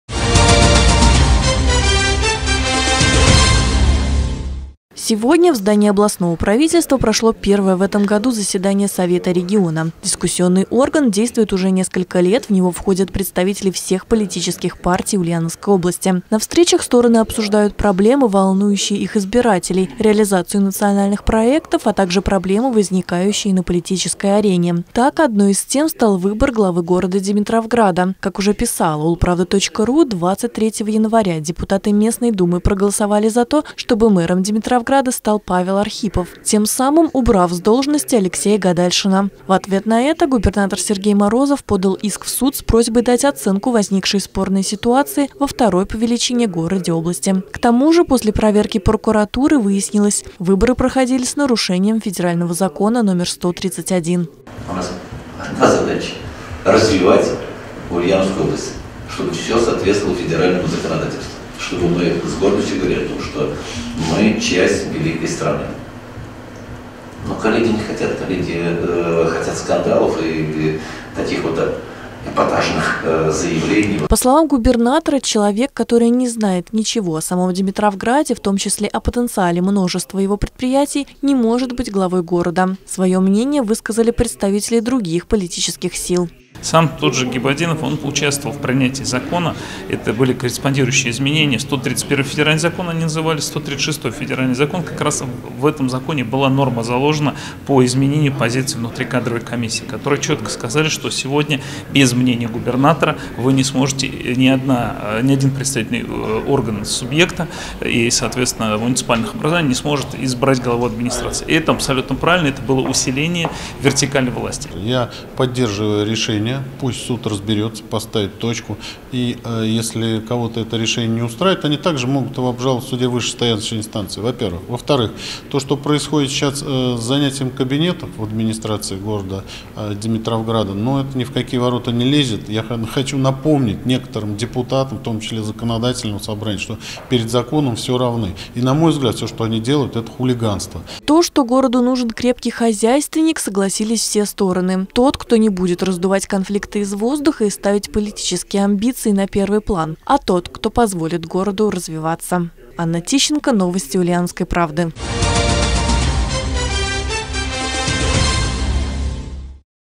Сергей Морозов: «У нас одна задача – развивать Ульяновскую область»
Сегодня в здании областного правительства прошло первое в этом году заседание совета региона. Дискуссионный орган действует уже несколько лет, в него входят представители всех политических партий Ульяновской области. На встречах стороны обсуждают проблемы, волнующие избирателей, реализацию национальных проектов, а также проблемы, возникающие на политической арене.